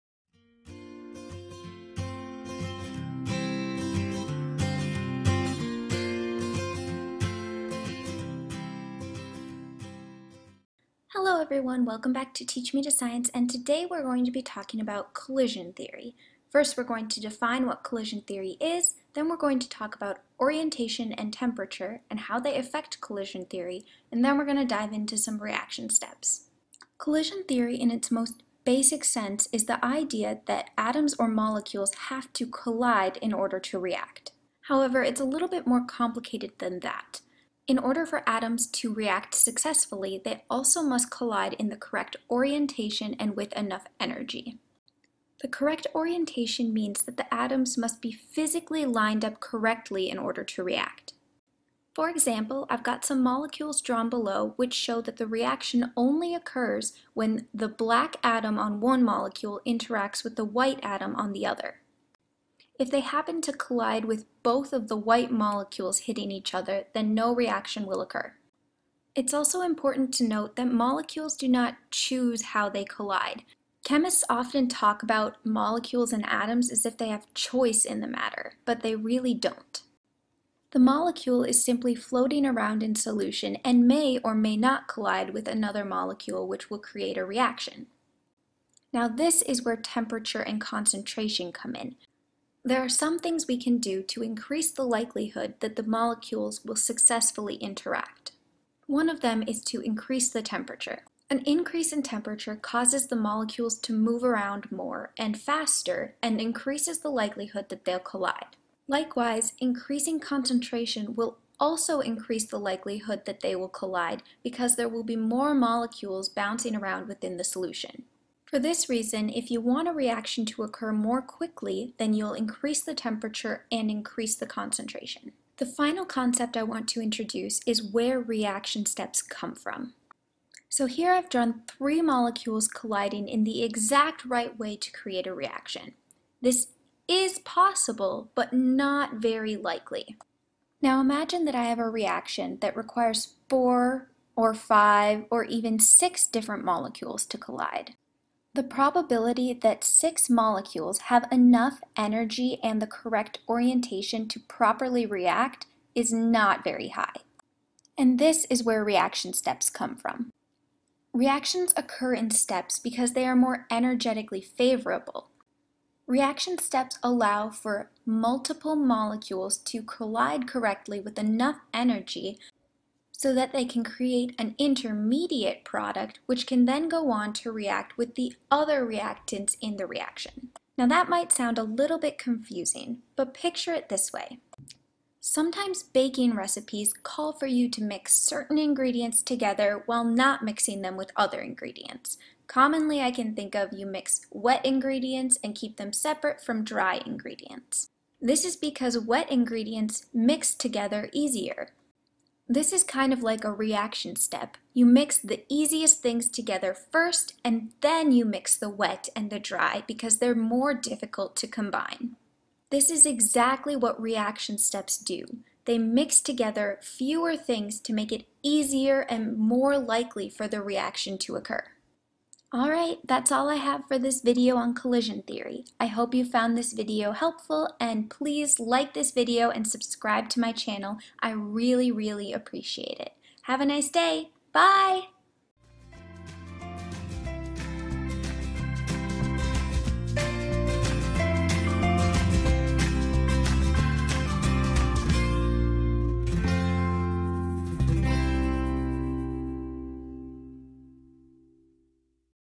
Collision Theory
What makes a reaction occur on the smallest level possible? How do molecules interact to make a reaction occur? In this video I'll talk about the molecular basis which causes reactions to occur and other important factors which influence a reaction.

I absolutely love teaching. However, part of teaching is being a lifelong learner.